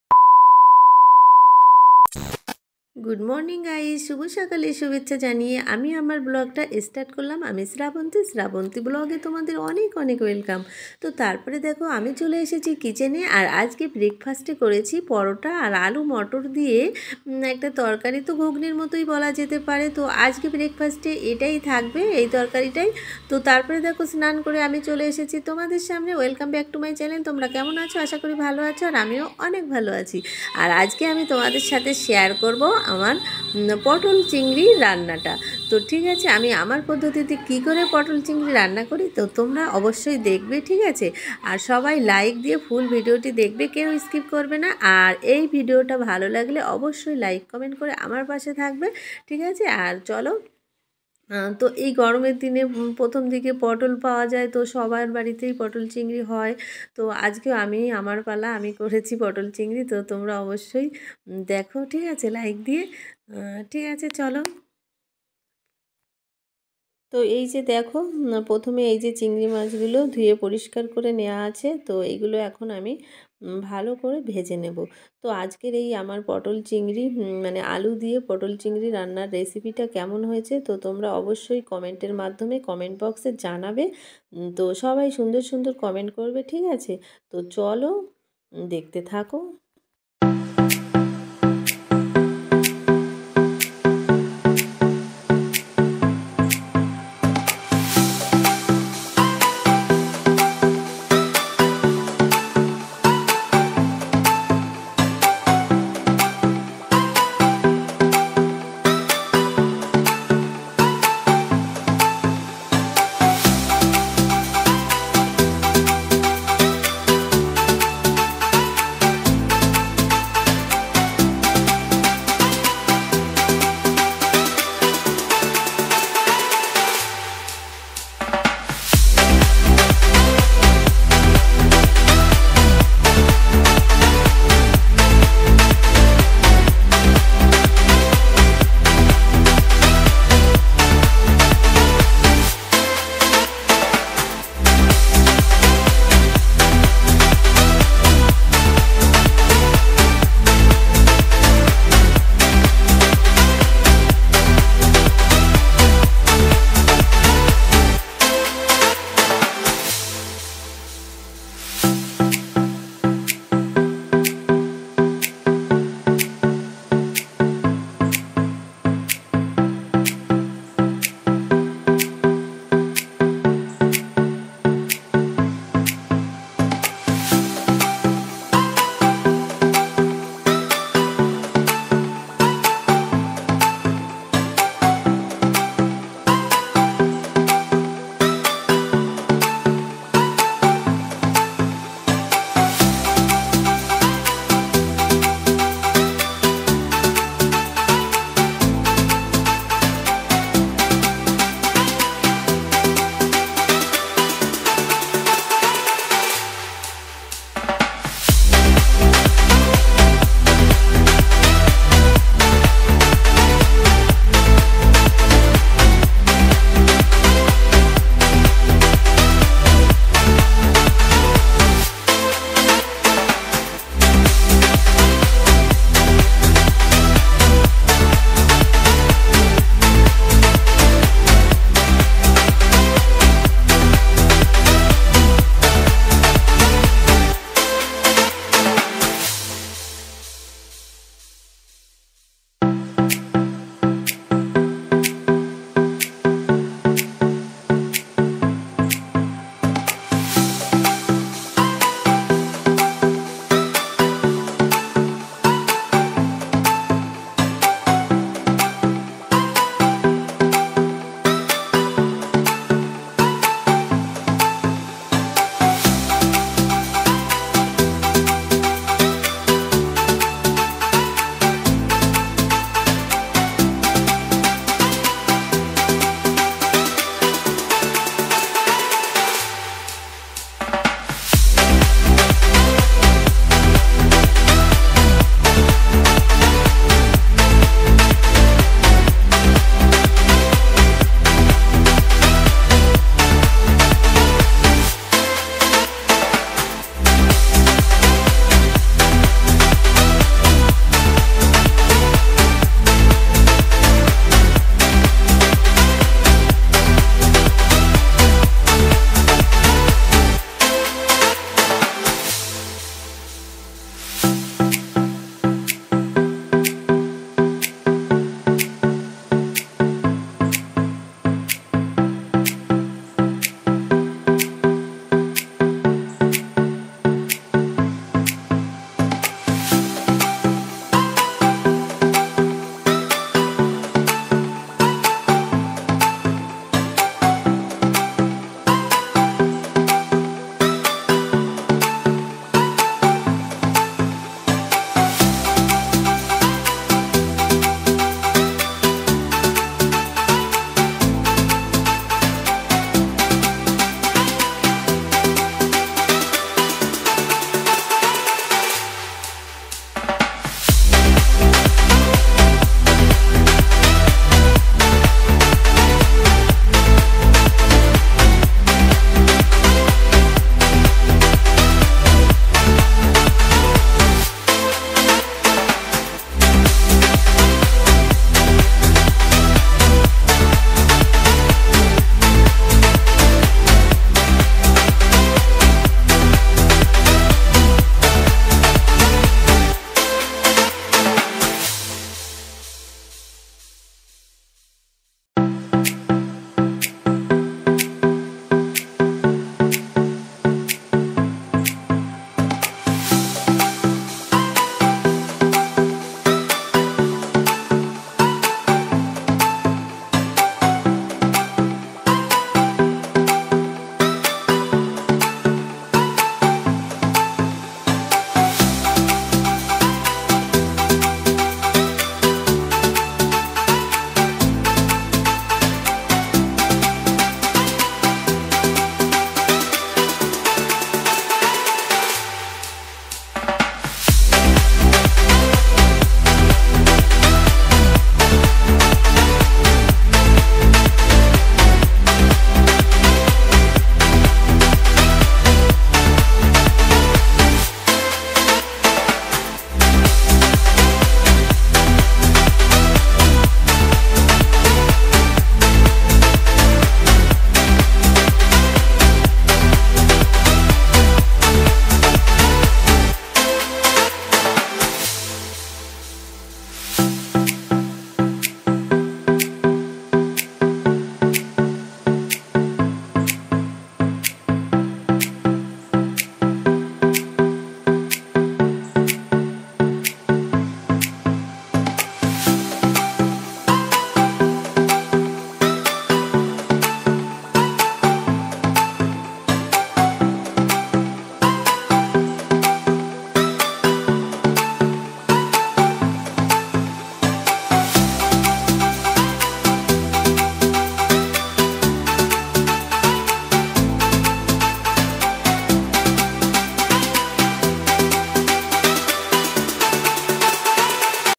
আজ আমি রান্না করেছি আলু পটল দিয়ে পটল চিংড়ি//potol chingri recipe//shrabanti blog #
আজ আমি রান্না করেছি আলু পটল দিয়ে পটল চিংড়ি//potol chingri recipe//

my daily blog ❤️

bengali blog